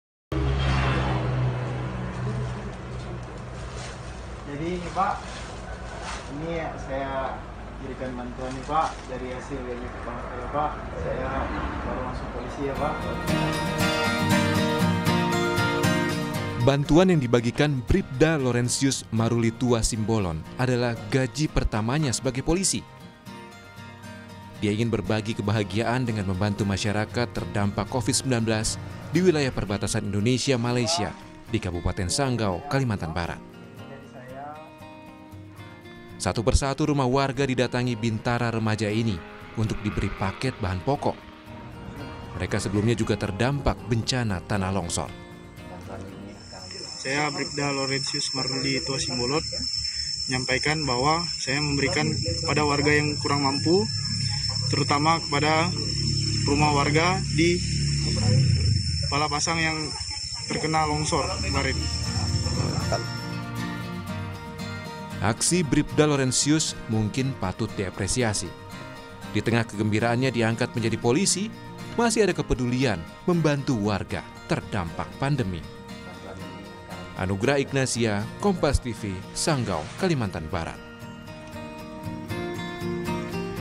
Polisi Ini Sumbangkan Gaji Pertama Untuk Terdampak Corona di Perbatasan Indonesia-Malaysia
SANGGAU, KOMPAS.TV - Seorang anggota polisi membagikan gaji pertamanya kepada warga terdampak Covid-19 di Kabupaten Sanggau, Kalimantan Barat.

Dia membelikan bahan pokok dari gaji pertama untuk warga di perbatasan Indonesia-Malaysia.

Bantuan yang dibagikan Bripda Lorensius Marulitua Simbolon adalah gaji pertamana sebagai polisi.

Dia ingin berbagi kebahagiaan dengan membantu masyarakat terdampak Covid-19 di wilayah perbatasan Indonesia  Malaysia di Kabupaten Sanggau, Kalimantan Barat.

Satu per satu rumah warga didatangi bintara remaja ini untuk diberi paket bahan pokok.

Mereka sebelumnya juga terdampak bencana tanah longsor.

Aksi Bripda Lorensius mungkin patut diapresiasi.

Di tengah kegembiraannya diangkat menjadi polisi, masih ada kepedulian membantu warga terdampak pandemi.